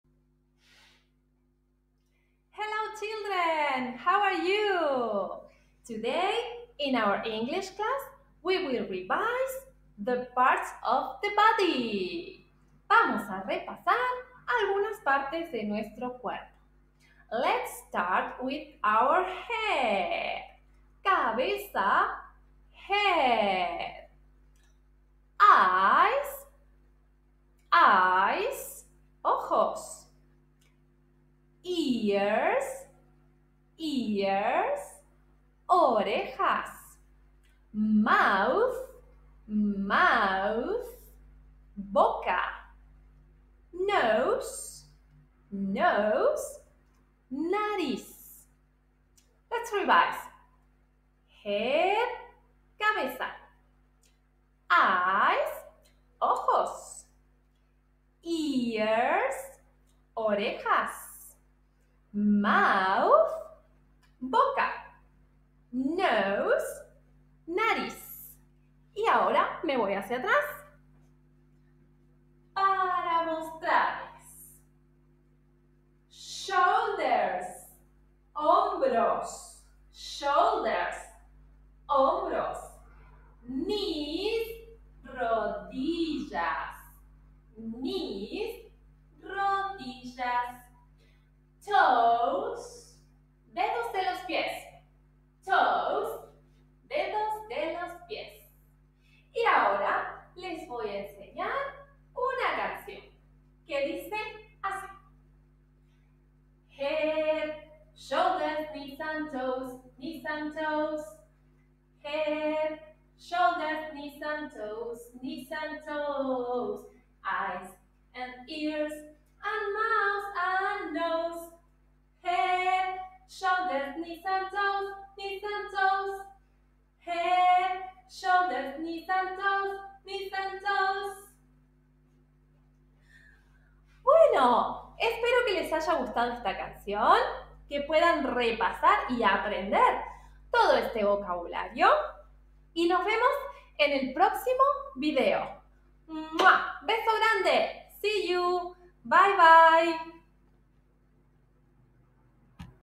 My body (Salita de 5 años)
La profe nos hace un repaso de las partes del cuerpo en inglés.